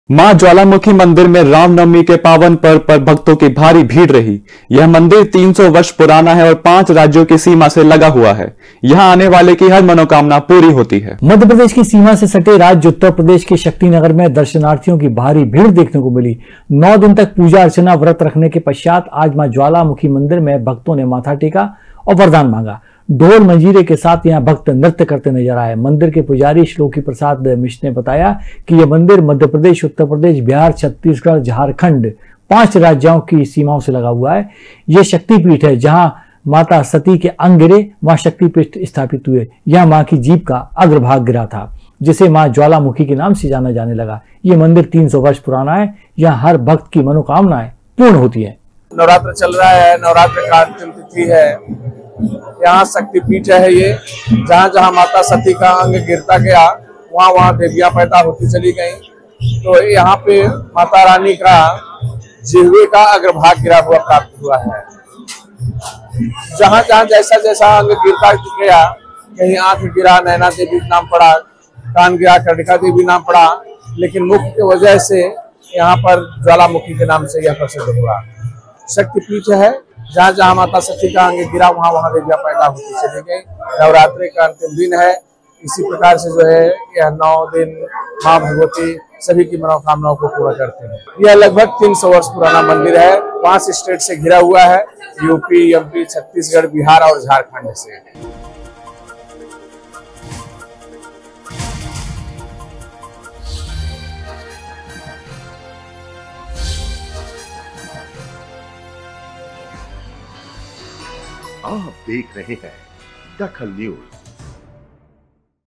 DAKHAL NEWS ज्वालामुखी मंदिर में भक्तों की लगी भीड़
ज्वालामुखी मंदिर में भक्तों  की लगी भीड़
 300 वर्ष  पुराना है  ज्वालामुखी मंदिर